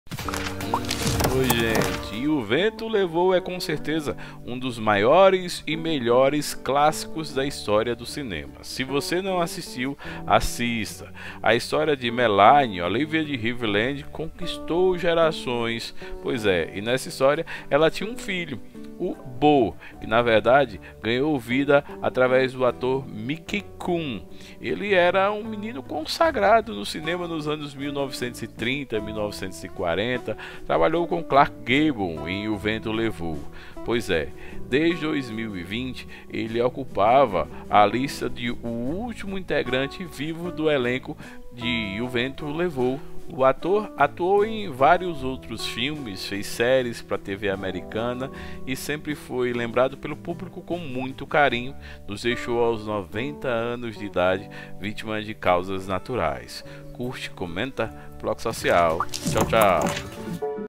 LUTO • Morre último ator vivo de um clássico, Sempre querido Mickey Kuhn de E o vento levou
Ele será imortal no cinema!

Mickey Kuhn, que como ator mirim marcou presença em vários filmes dos anos 1930 e 1940, morreu aos 90 anos de idade, de causas naturais. A informação foi confirmada pela esposa de Kuhn ao THR na noite de ontem (21).

Desde a morte de Olivia de Havilland, em 2020, Kuhn ocupava o posto de último integrante vivo do elenco do clássico ... E o Vento Levou, de 1939. No épico de Victor Fleming, ele interpretou Beau, o filho de Melanie (Havilland) e Ashley (Leslie Howard).

Outros créditos notáveis do ator incluem Juarez (1939), com Bette Davis; uma versão de Dick Tracy (1945) estrelada por Morgan Conway; o faroeste Rio Vermelho (1947), com John Wayne; e Uma Rua Chamada Pecado (1951), no qual se reuniu com sua colega de ... E o Vento Levou, Vivien Leigh.

Kuhn desistiu de Hollywood após algumas participações na série de TV Alfred Hitchcock Apresenta, em 1957. Ele trabalhou como gerente de aeroporto para a American Airlines por décadas antes de se aposentar, em 1995, e deixa a esposa, dois filhos e uma neta.

SIGA O APRESENTADOR: 👉 INSTAGRAM: @RayhBloom